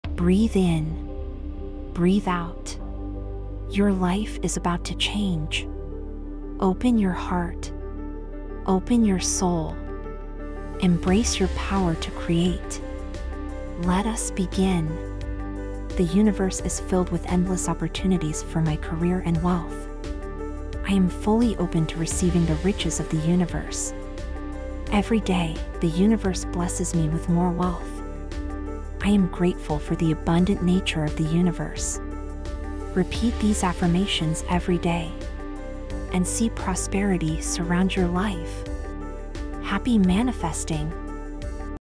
Day 4: Embracing the Universe's Riches
Title: "Day 4: Embracing the Universe's Riches | WealthAffirmations"

Description:
"Welcome to Day 4 of our WealthAffirmations journey! 🚀 Today, we're opening our hearts and minds to the universe's abundant riches. 💎✨

Our affirmations for today:

"The universe is filled with endless opportunities for my career and wealth." 🌌💼
"I am fully open to receiving the riches of the universe." 🙌💰
"Every day, the universe blesses me with more wealth." 🌞💵
"I am grateful for the abundant nature of the universe." 🙏🌟

Let's embrace the abundant riches of the universe together! 🌈💫"